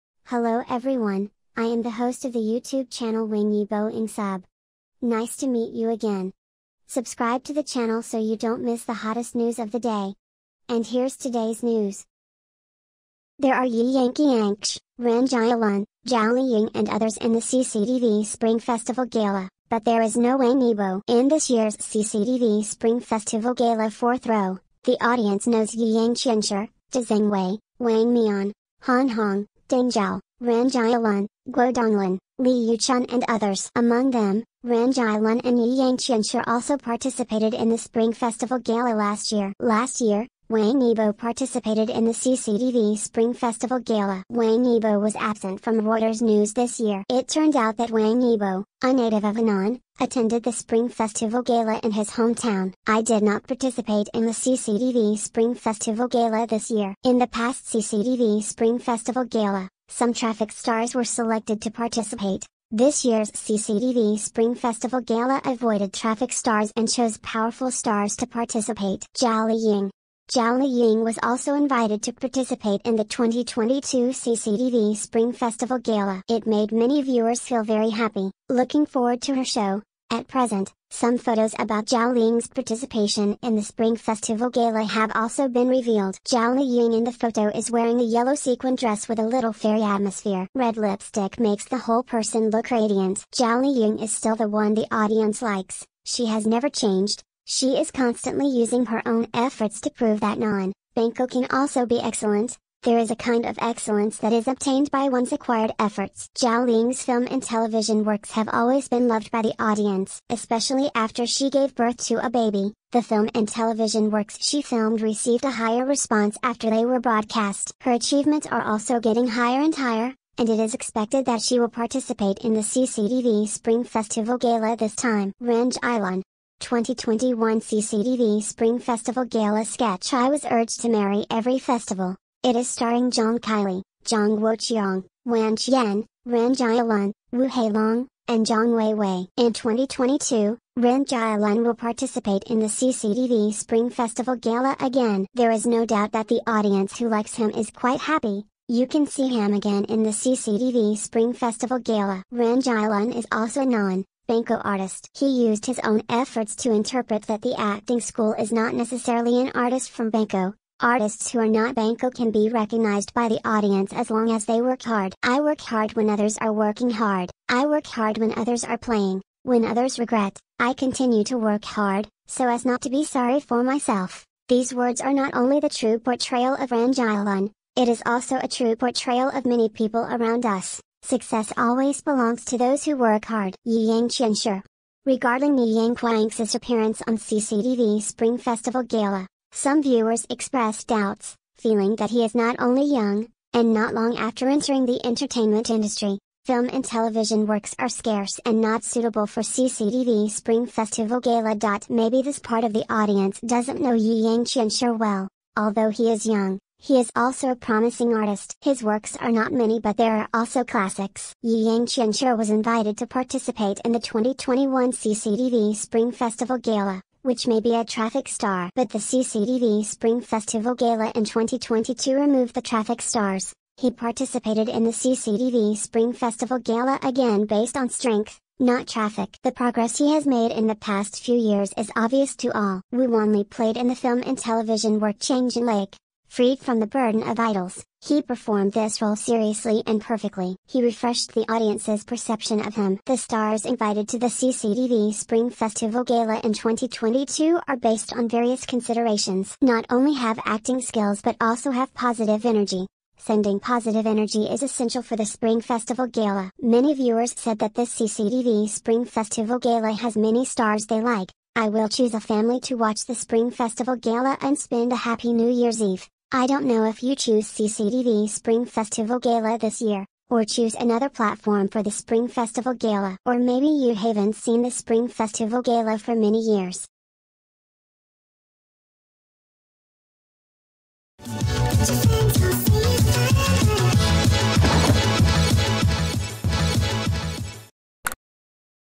There are Yi Yangqianxi, Ren Jialun, Zhao Liying and others in the CCTV Spring Festival Gala, b...
WANG YIBO ENGSUB 2022/01/22:  There are Yi Yangqianxi, Ren Jialun, Zhao Liying and others in the CCTV Spring Festival Gala, but there is no Wang Yibo? .  In this year's CCTV Spring Festival Gala fourth row,  The audience knows Yi Yang Qianxi, Da Zhangwei, Wang Mian,  Han Hong, Deng Chao, Ren Jialun, Guo Donglin, Li Yuchun and others.  Among them, Ren Jialun and Yi Yang Qianxi also participated in the Spring Festival Gala last year.  Last year, Wang Yibo participated in the CCTV Spring Festival Gala.  Wang Yibo was absent from Reuters news this year.  It turned out that Wang Yibo, a native of Henan, attended the Spring Festival Gala in his hometown.  I did not participate in the CCTV Spring Festival Gala this year.  In the past CCTV Spring Festival Gala, some traffic stars were selected to participate,  This year's CCTV Spring Festival Gala avoided traffic stars and chose powerful stars to participate.  Zhao Liying. Zhao Liying was also invited to participate in the 2022 CCTV Spring Festival Gala.  It made many viewers feel very happy, looking forward to her show,  At present, some photos about Zhao Liying's participation in the Spring Festival Gala have also been revealed.  Zhao Liying in the photo is wearing a yellow sequined dress with a "little fairy" atmosphere.  Red lipstick makes the whole person look radiant.  Zhao Liying is still the one the audience likes, she has never changed,  She is constantly using her own efforts to prove that non-banco can also be excellent,  There is a kind of excellence that is obtained by one's acquired efforts.  Zhao Liying's film and television works have always been loved by the audience.  Especially after she gave birth to a baby, the film and television works she filmed received a higher response after they were broadcast.  Her achievements are also getting higher and higher, and it is expected that she will participate in the CCTV Spring Festival Gala this time.  Ren Jialun. 2021 CCTV Spring Festival Gala sketch "I was urged to marry every festival",  I